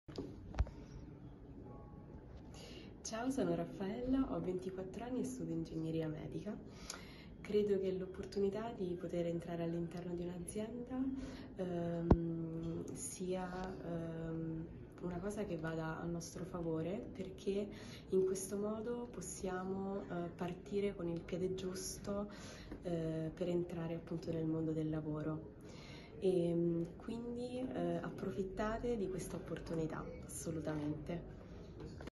Raffaella Ricci, studentessa di Ingegneria Medica, Università di Tor Vergata
Siamo ospiti di Italgas per un del'anteprima di People Power Talent Tour: studenti e laureati partecipano a una giornata di orientamento alle carriere e di talent scouting insieme ai manager di Italgas (Roma, 15 giugno 2023).